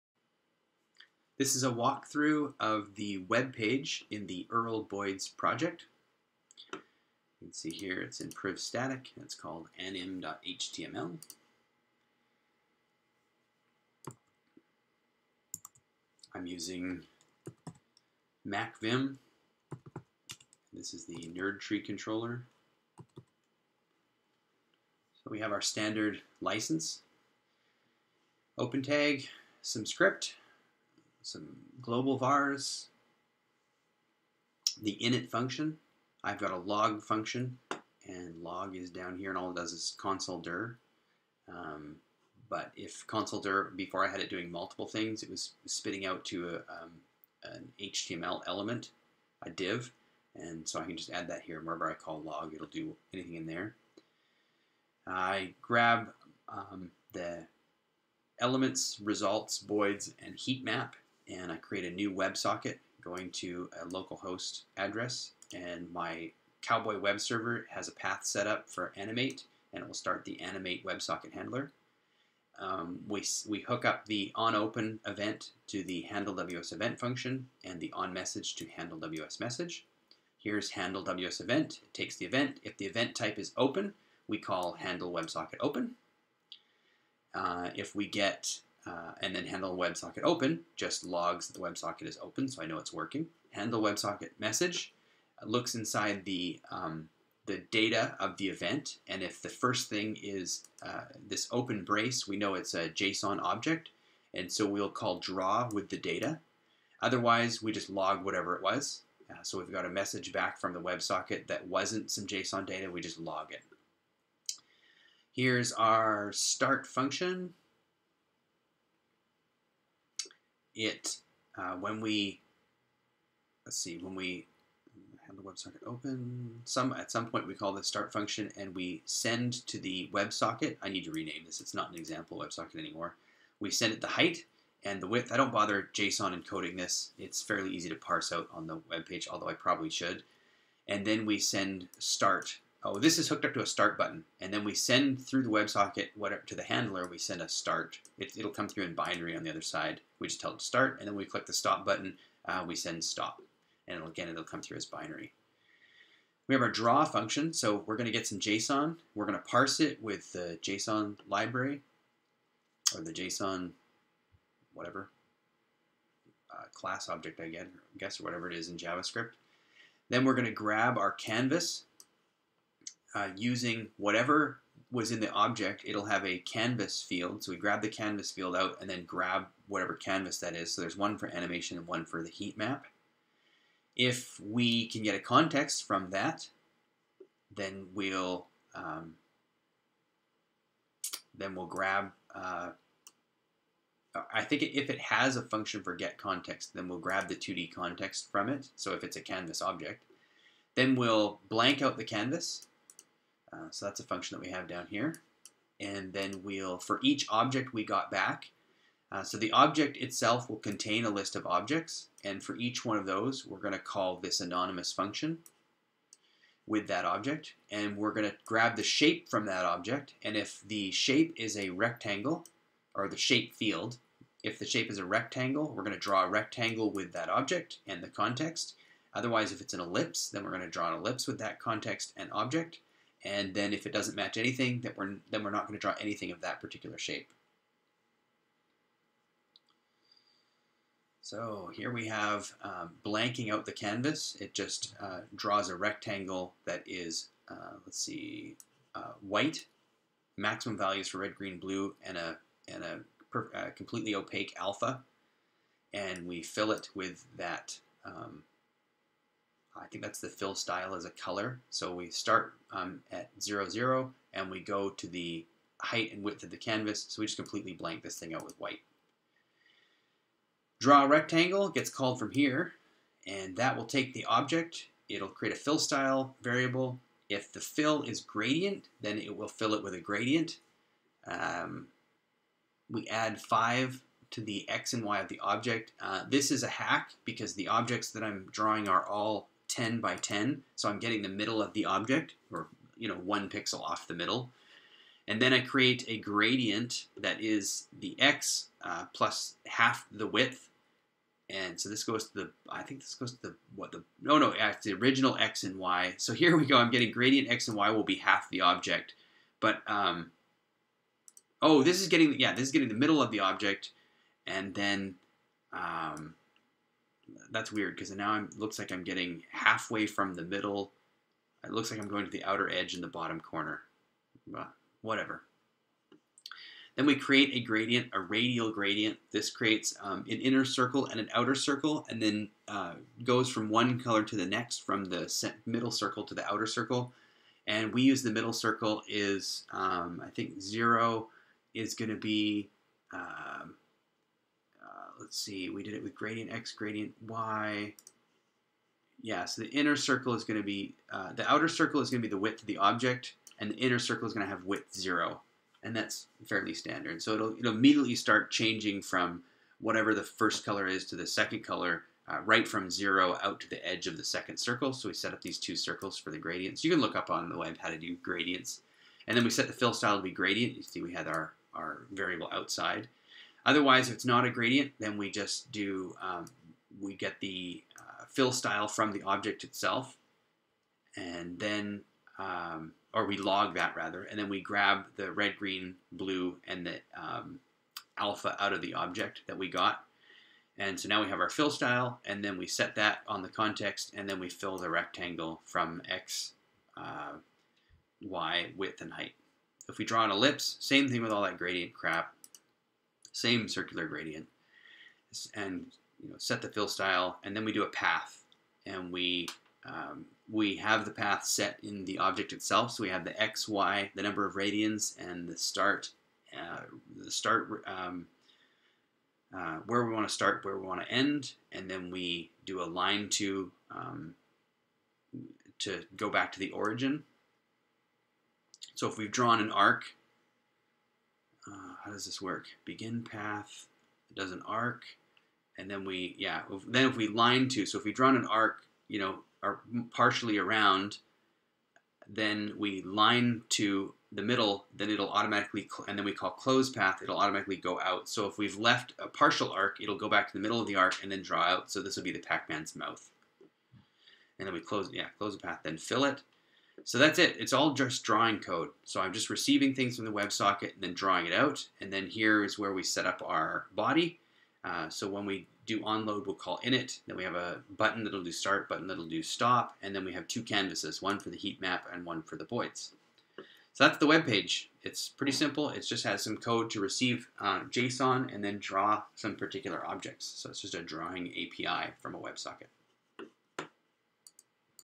erl_boids animate webpage (priv/static/anim.html)
Walkthrough of the web page that receives JSON drawing objects from a websocket and draws them with the specified shape, gradient, colour, alpha and cavas.